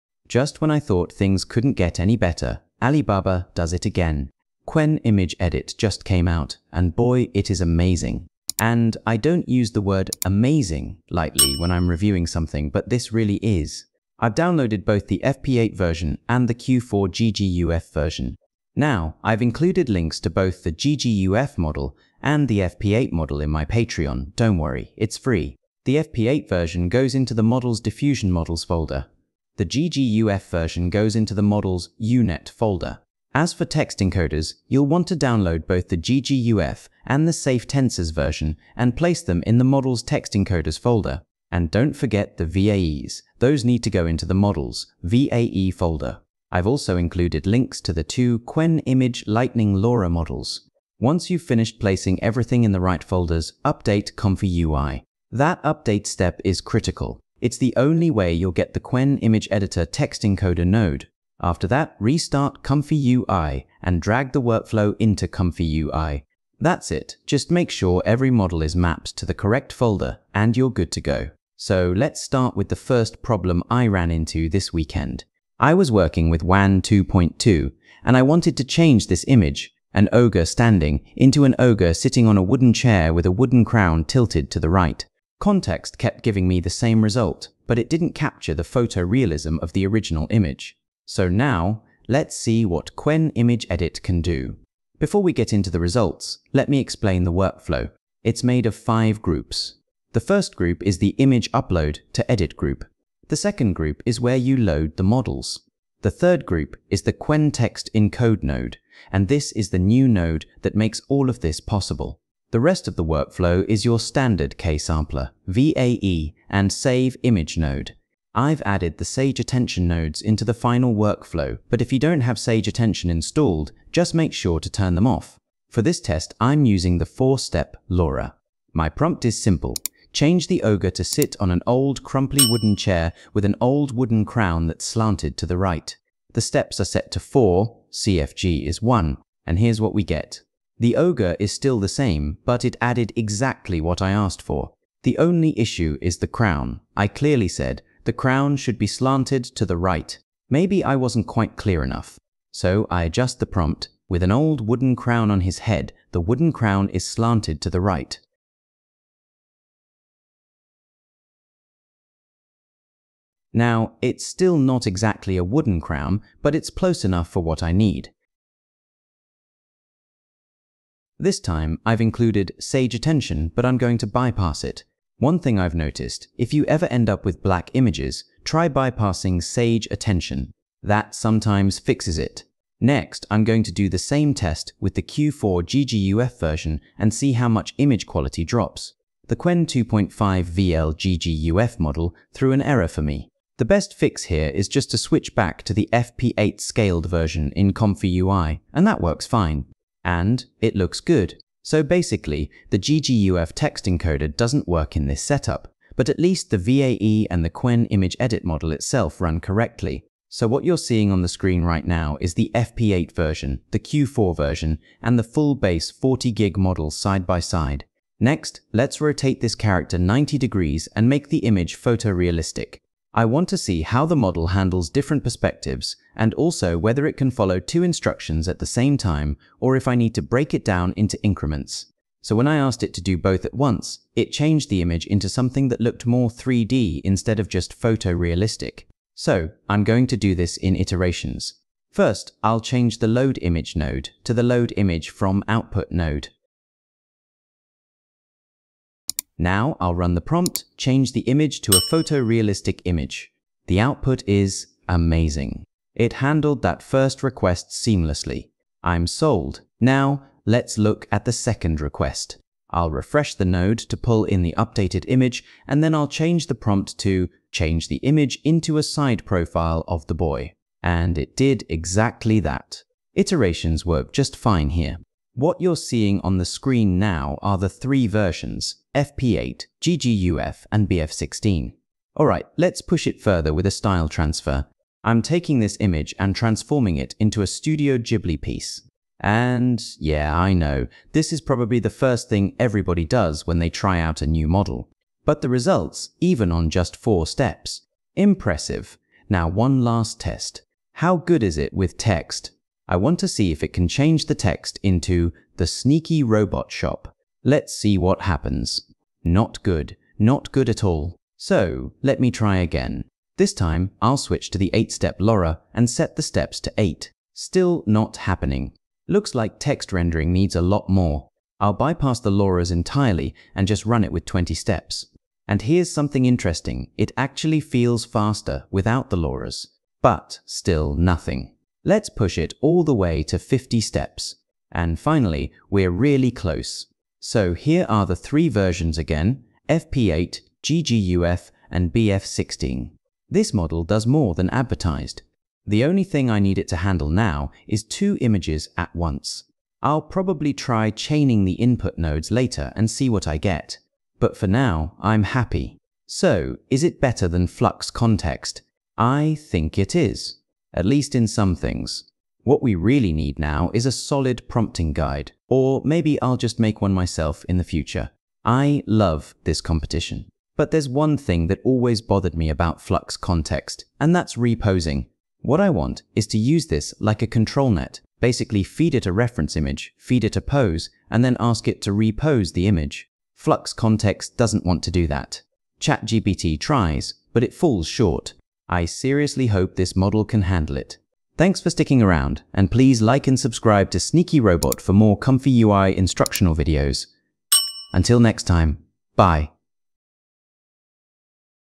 Qwen Image Editing First Look in ComfyUI With Free Workflows… and it’s seriously next level
Qwen Image Edit New AI workflow in ComfyUI that actually delivers photorealistic edits.

0:00 intro
0:14 Quick setup
1:11 Start Test And Workflow
05:45 Verdict